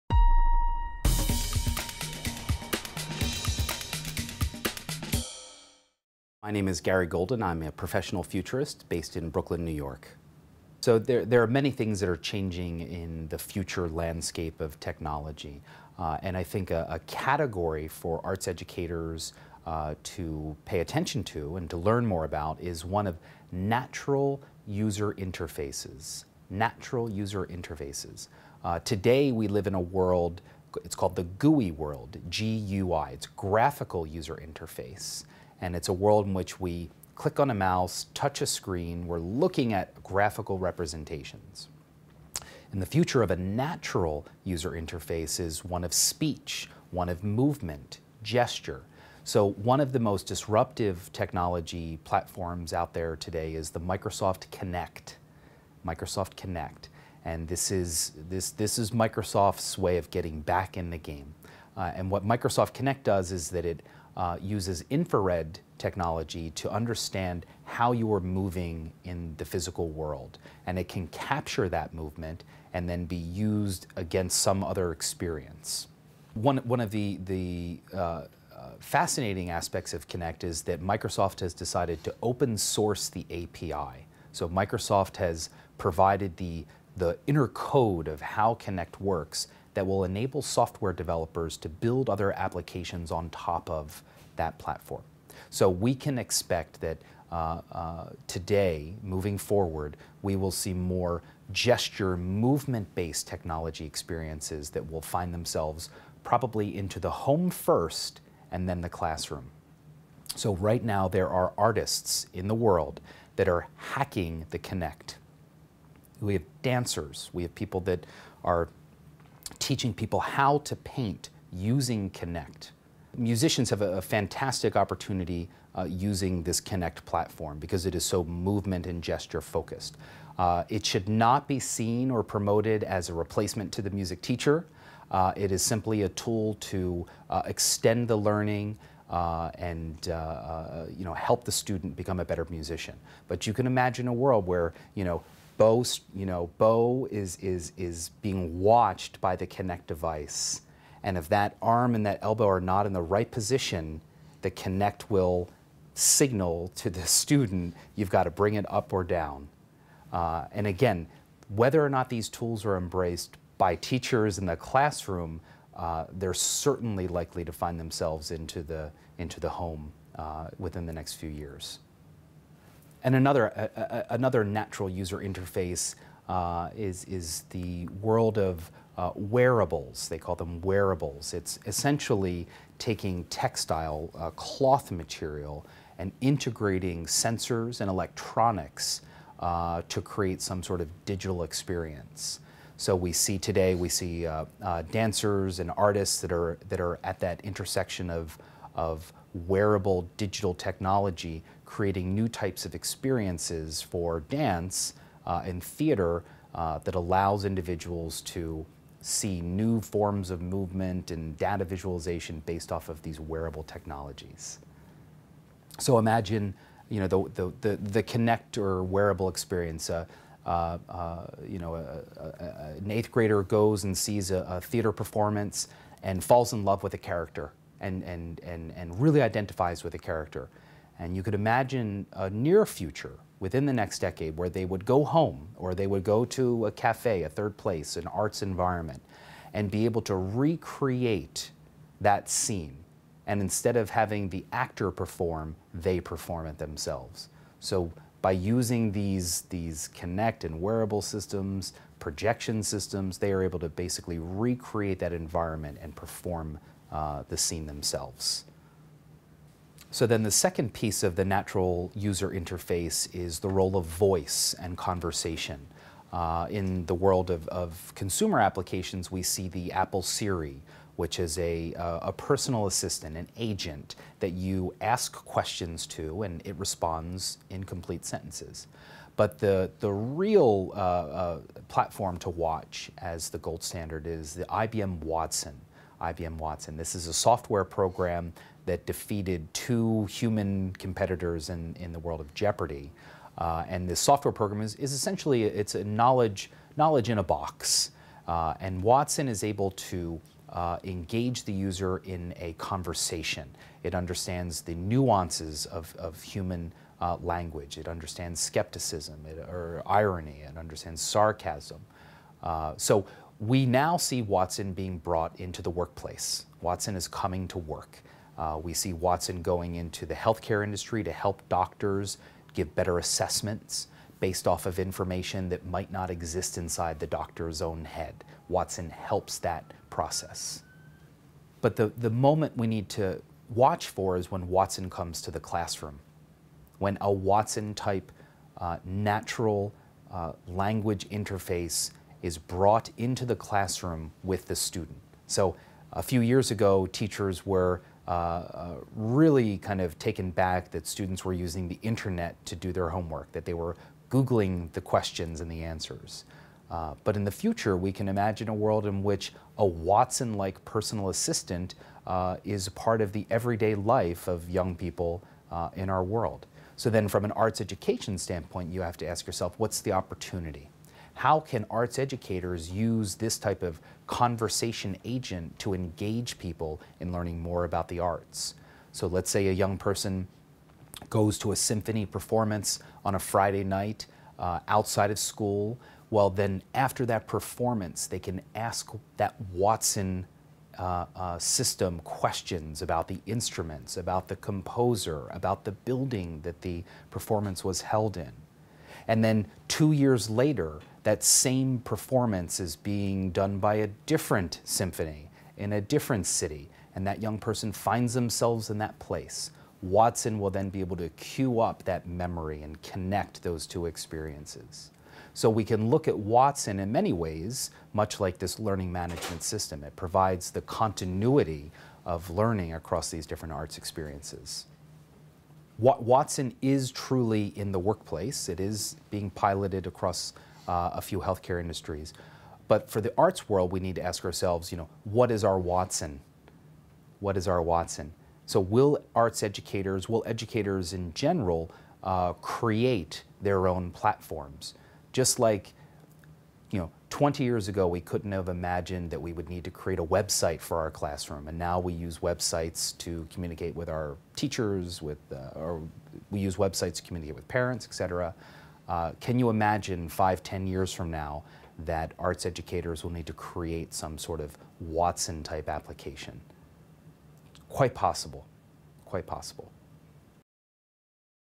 Garry Golden on Technology (5 of 5)
Garry Golden is a professionally trained Futurist who writes, speaks and consults on issues shaping business and society in the 21st century. In the final part of this interview, Garry looks ahead to the future of how we will interact with technology using natural user interfaces, like Microsoft's Kinect, Apple's Siri, and IBM's Watson. He talks about how movement-based interfaces like Kinect are rapidly finding their way into arts education, and how conversation-based personal assistants like Watson are already having an impact in the workplace (and the future they offer for educators).